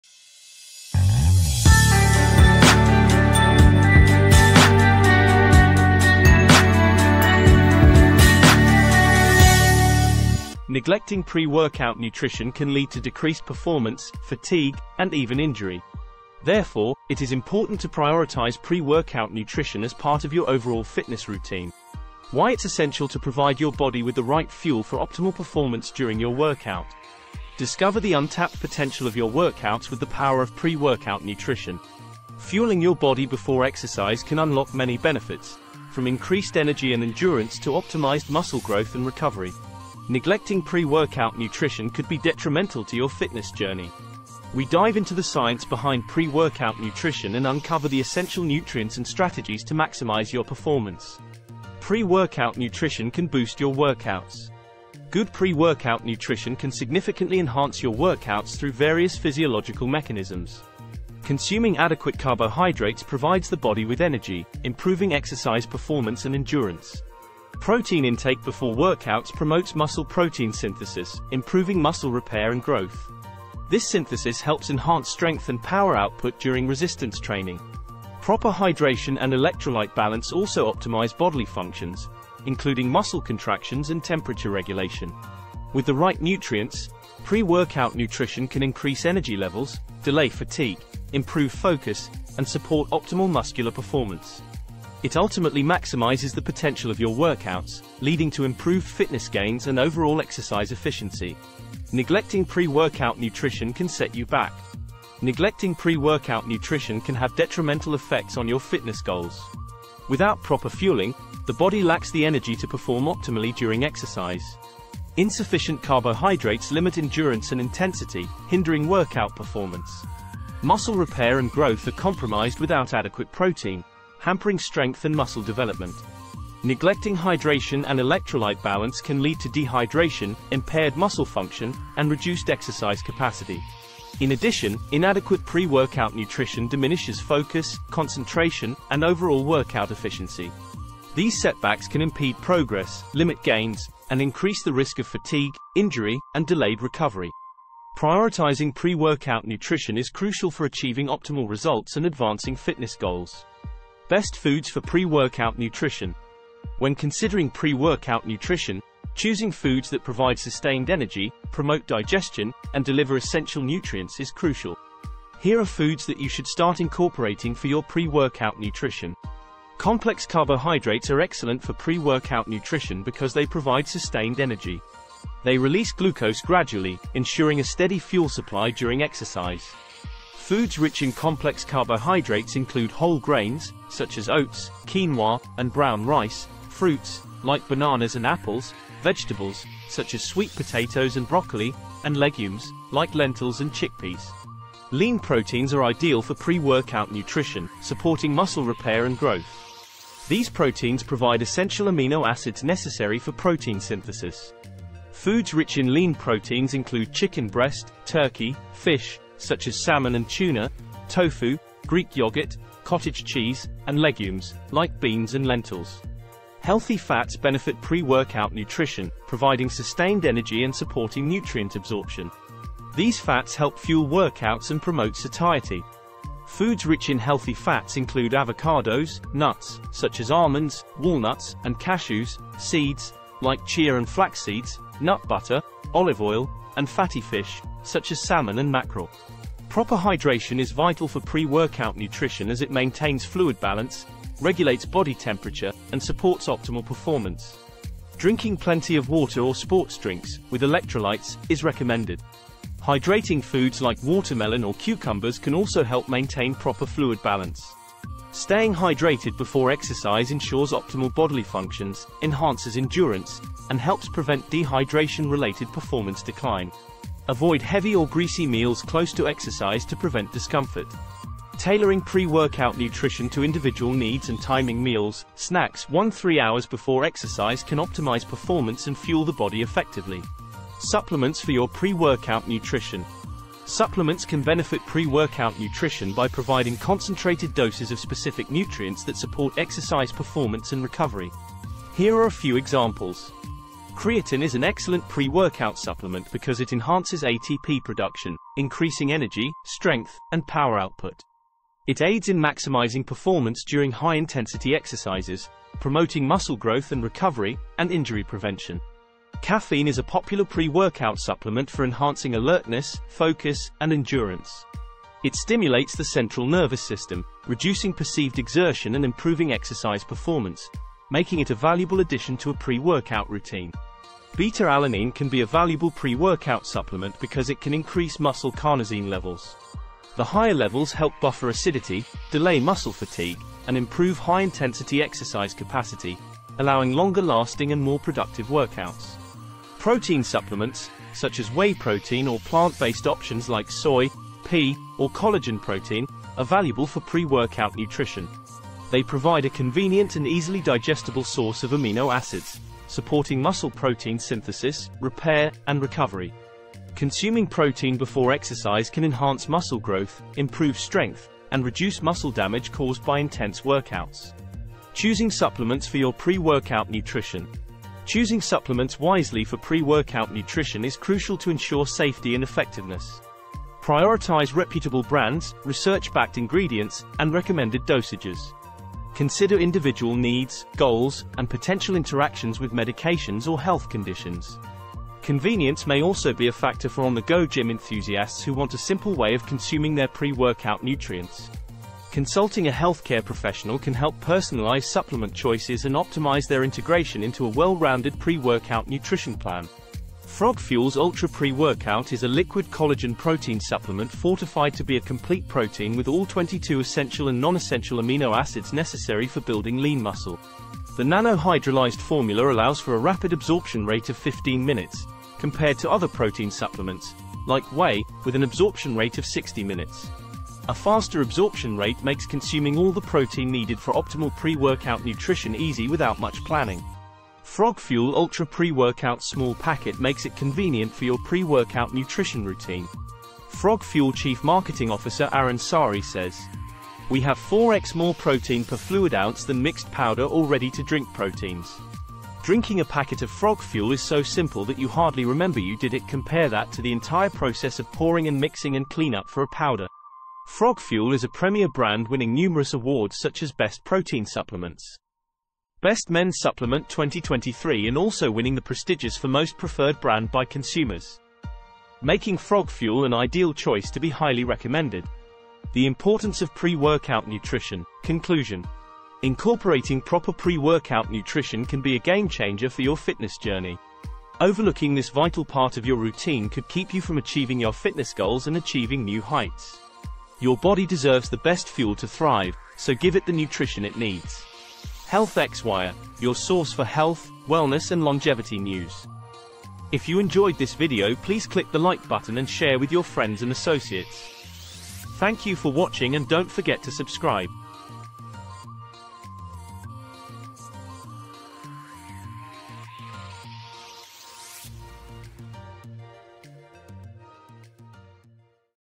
Fueling Your Workout The Importance of Pre Workout Nutrition
Neglecting pre-workout nutrition can harm performance, cause fatigue, and increase injury risk. Prioritize it for fitness success. Discover how pre-workout nutrition enhances energy, endurance, muscle growth, and recovery. It's vital for maximizing workouts. Inadequate nutrition hampers exercise potential, leading to setbacks and increased risk of fatigue, injury, and delayed recovery.

Optimal pre-workout foods include complex carbohydrates, lean proteins, and healthy fats, while supplements like creatine, caffeine, and beta-alanine can boost performance. Choose supplements carefully, considering safety, goals, and individual needs.

Frog Fuel's Ultra Pre-Workout is a convenient option. Don't underestimate the importance of pre-workout nutrition for your fitness journey. Give your body the nutrition it deserves to thrive.